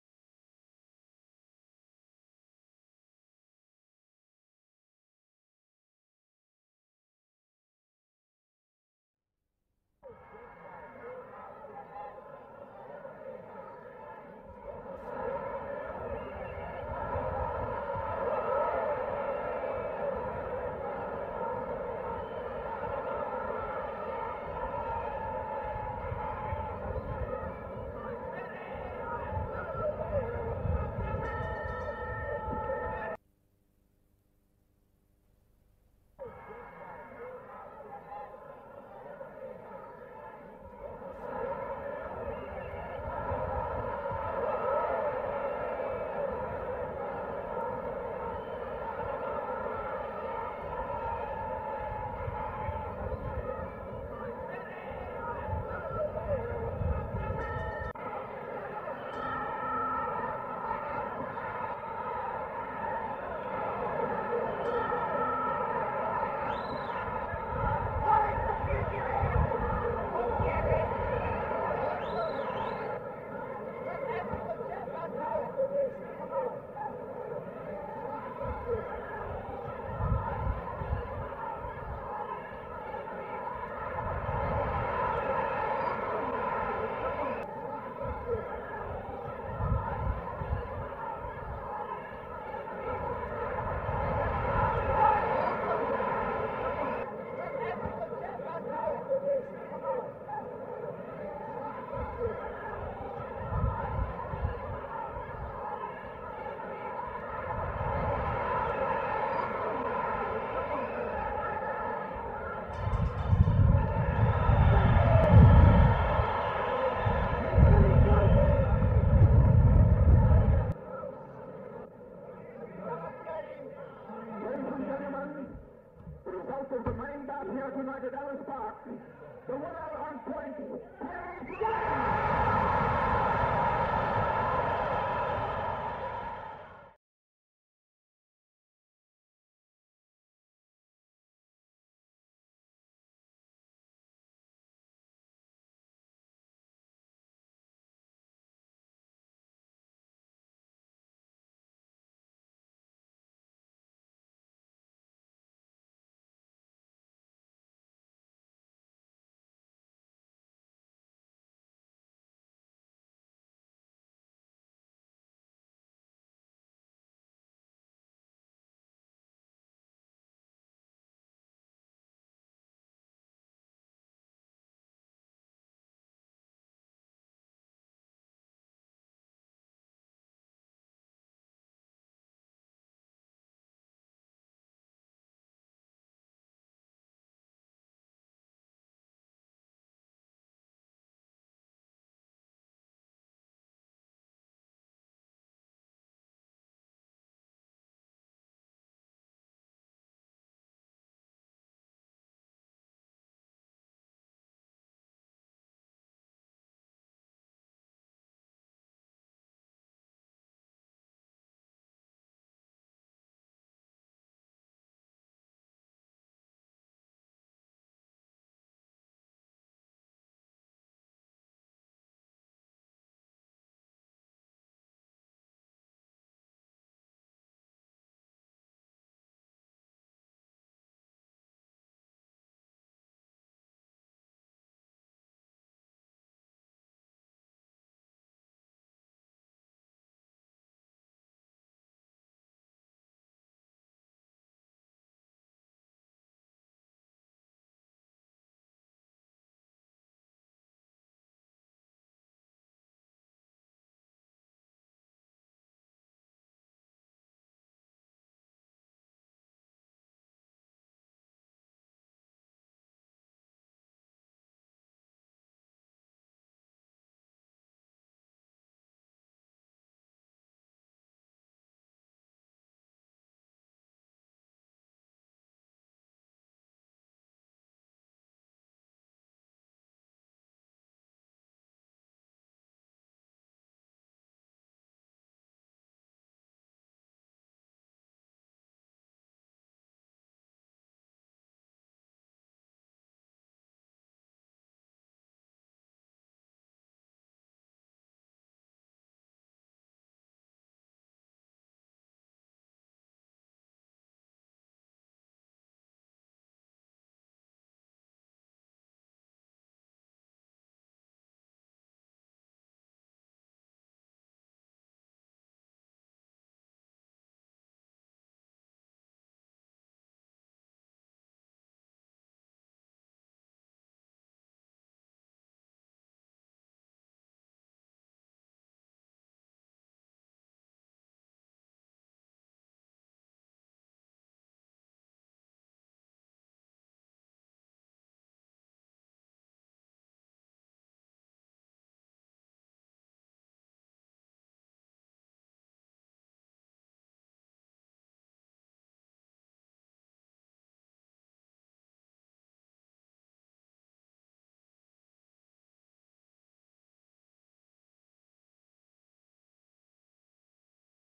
Pierre Fourie vs Mike Quarry, Widescreen Match Highlights & Slow motion 1974, Fourie with red belt
ighters: Pierre Fourie (South Africa, red belt) vs Mike Quarry (USA, white belt)
Category: Light Heavyweight, 10 rounds
Date: Saturday March 2, 1974
Location: Johannesburg, South Africa
Outcome: Fourie wins by points